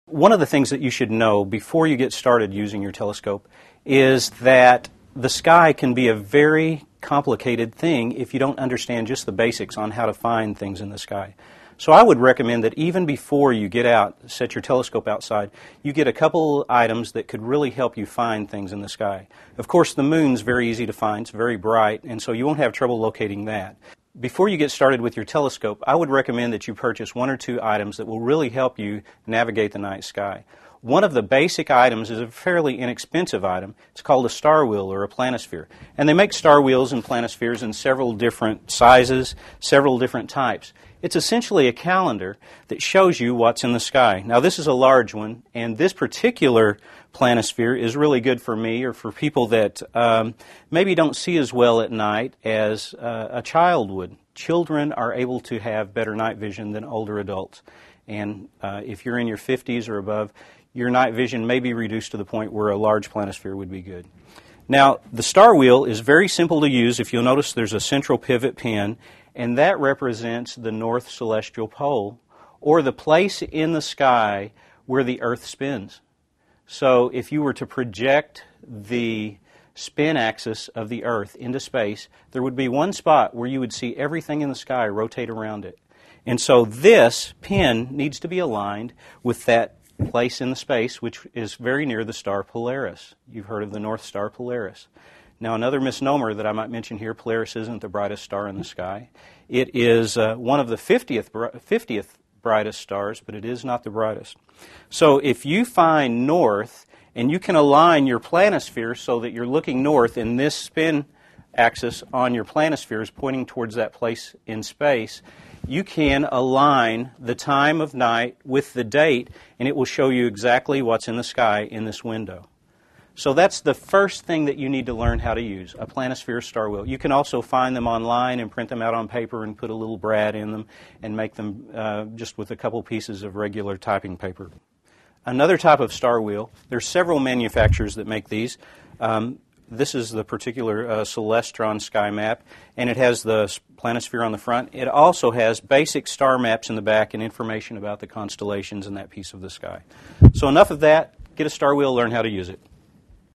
Using Star Charts With Telescopes
Using Star Charts With Telescopes. Part of the series: How to Use Telescopes. Star charts and star wheels help home telescope users find and identify stars in the night sky. Learn to use star charts for telescope viewing from an observatory director in this free astronomy video.